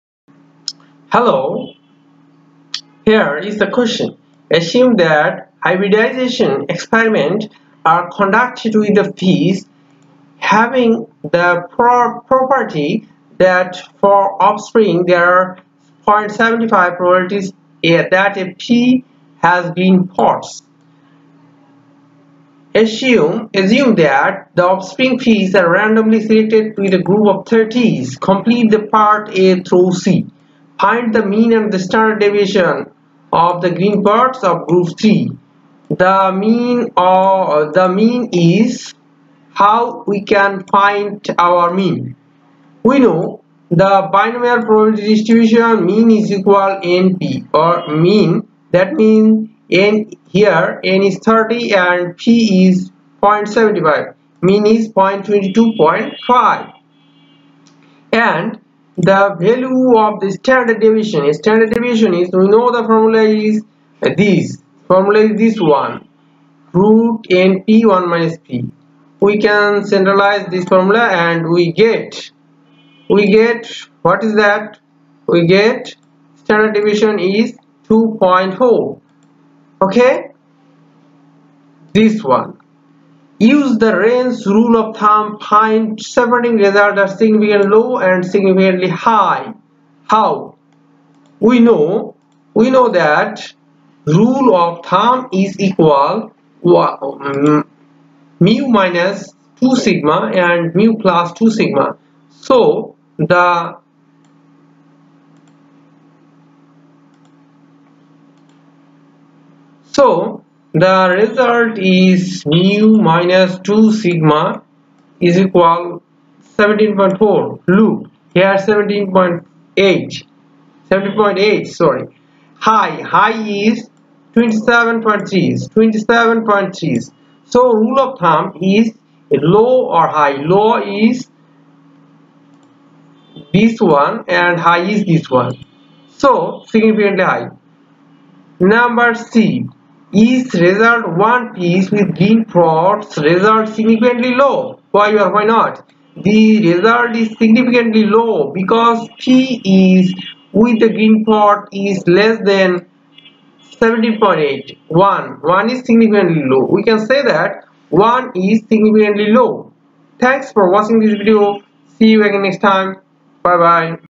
53. Assume that hybridization experiments are conducted with peas having the property that for
53.
Assume that hybridization experiments are conducted with peas having the property that for offspring, there is a 0.75 probability that a pea has green pods. Assume that the offspring peas are randomly selected in groups of 30.

a. Find the mean and the standard deviation for the numbers of peas with green pods in the groups of 30.
The value of the mean is µ= 22.5 peas.

The value of the standard deviation is s= 2.4 peas.

b. Use the range rule of thumb to find the values separating results that are significantly low or significantly high.

Values of 17.8 peas or fewer are significantly low.

Values of  27.2 peas or greater are significantly high.

c. Is a result of 1 peas with green pods a result that is significantly low? Why or why not?
The result is significantly low, because 1 peas with green pods is less than 17.8 peas.

Note: this video is make only for Education.